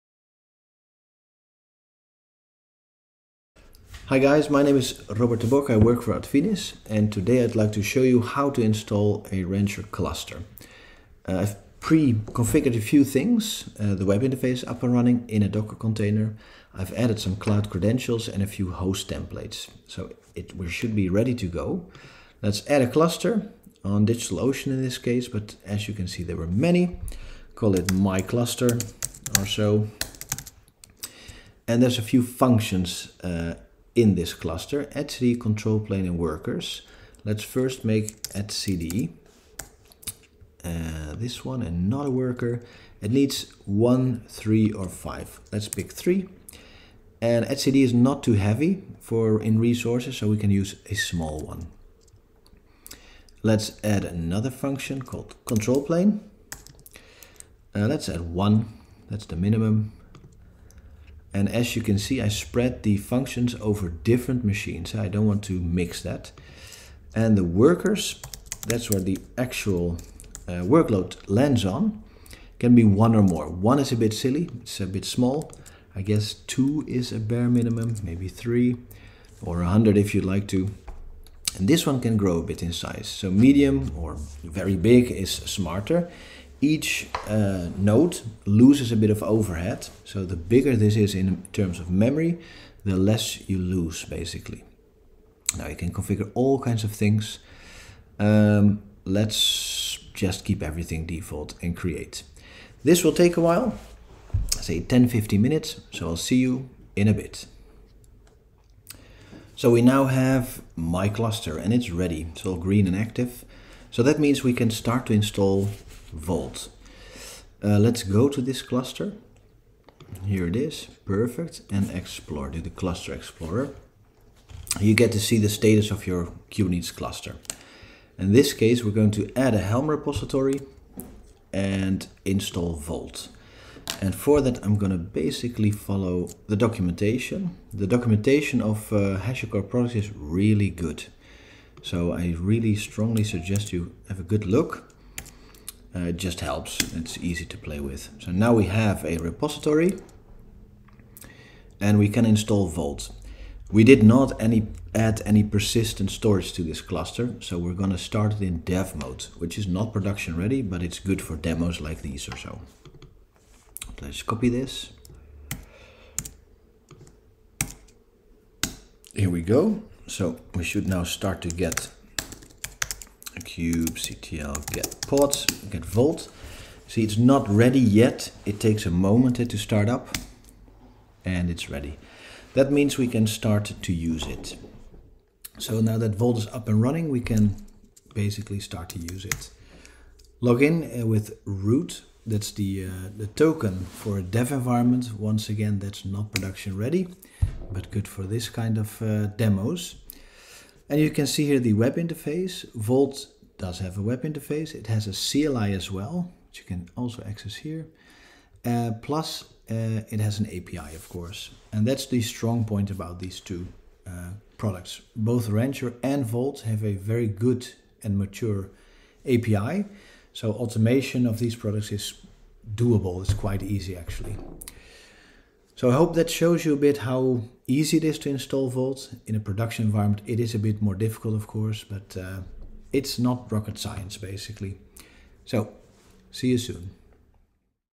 Rancher + Vault
Here is a demo on how to create a K8s cluster using Rancher and install Vault in the cluster.